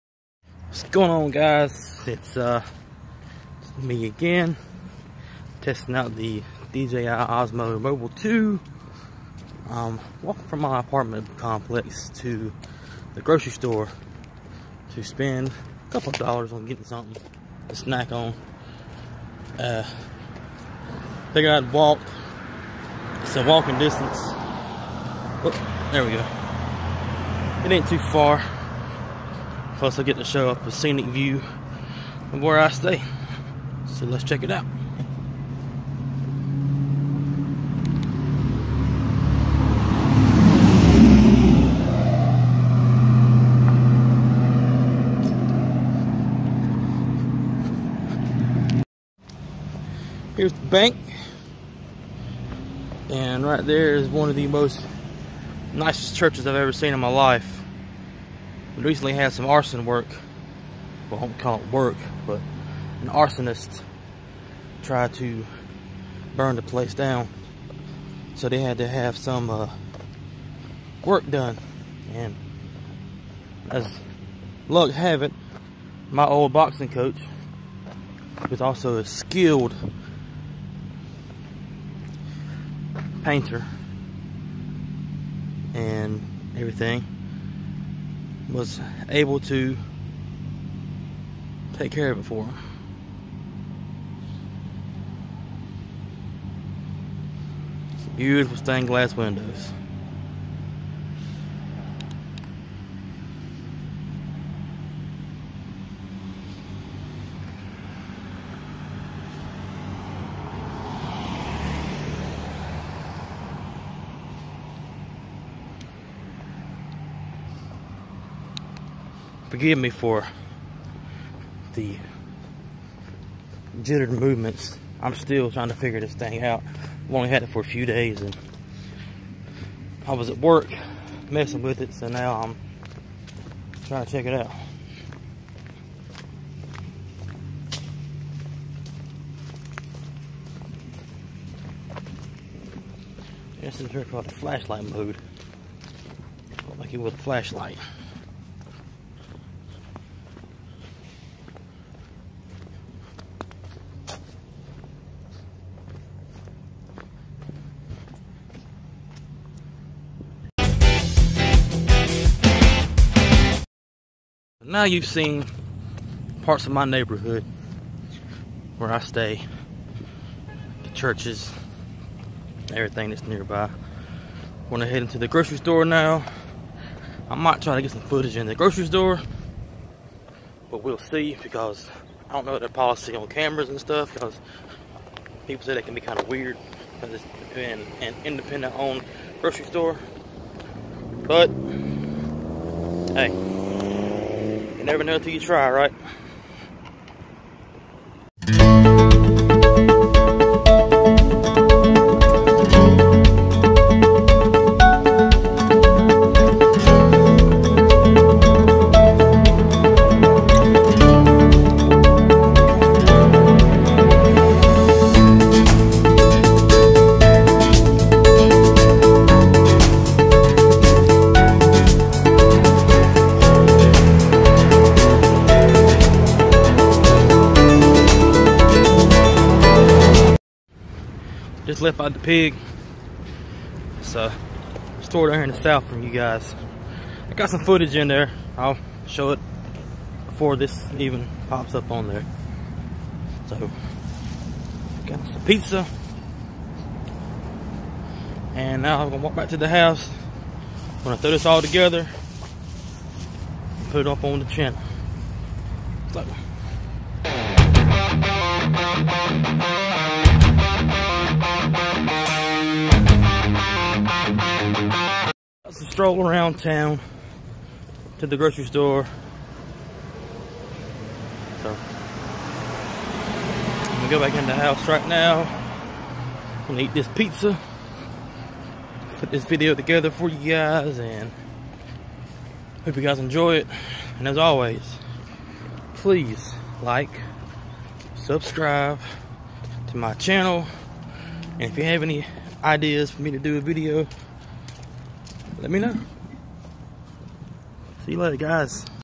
DJI Osmo Mobile 2/ trek around town
With the new DJI Osmo Mobile 2, I walked around my neighborhood and filmed various places